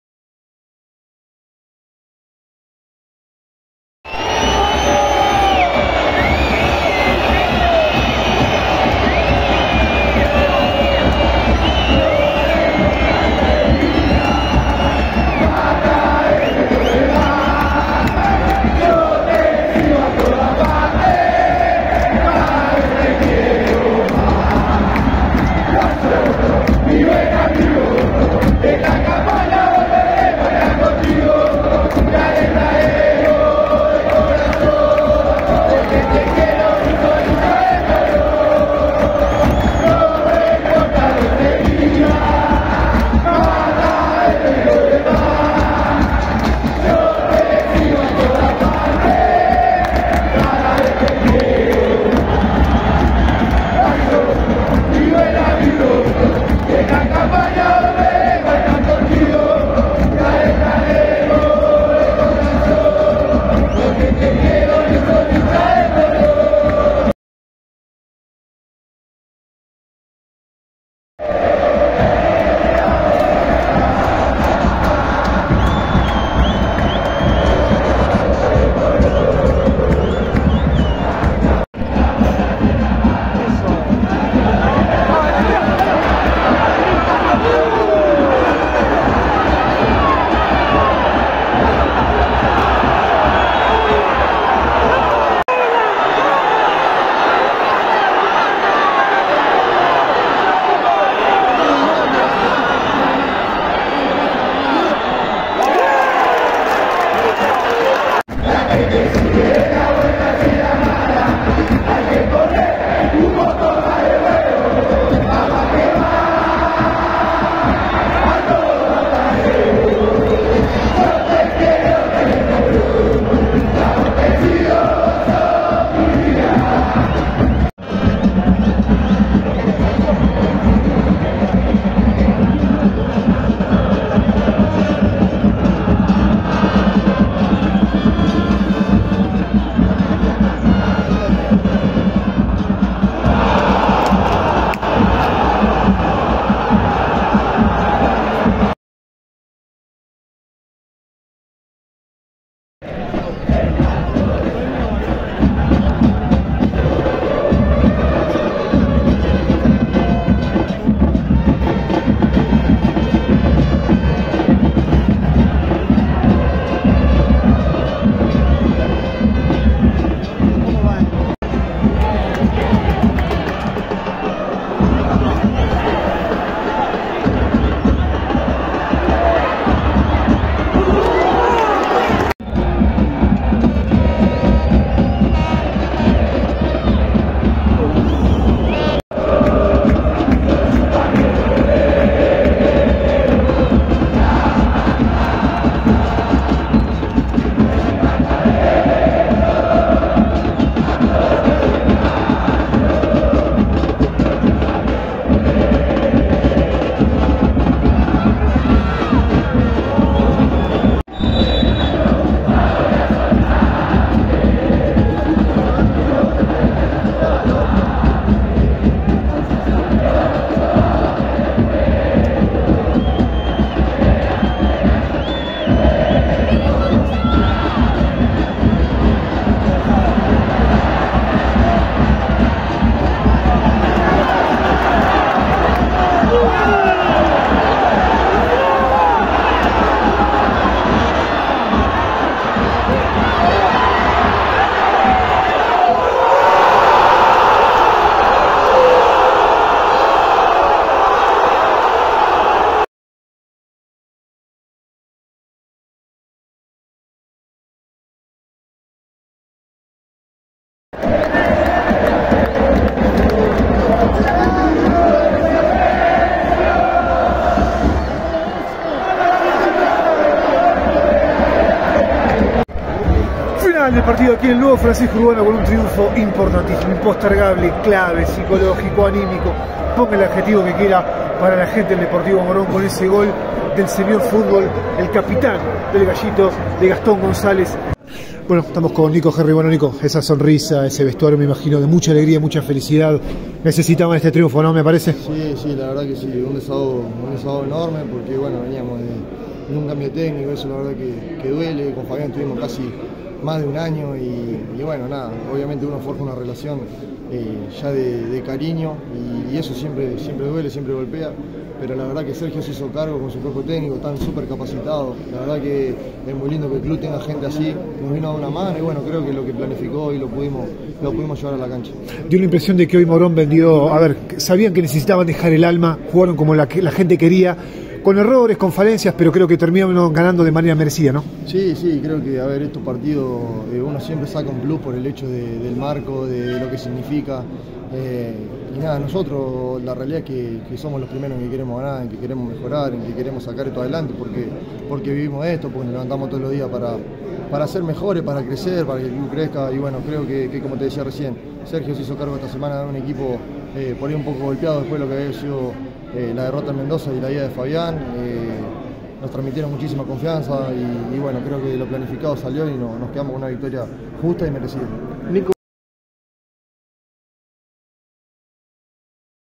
La película del clásico entre Deportivo Morón y Nueva Chicago
La tarde en el Nuevo Francisco Urbano dejó al Gallito como ganador del duelo ante el Torito con gol de Gastón González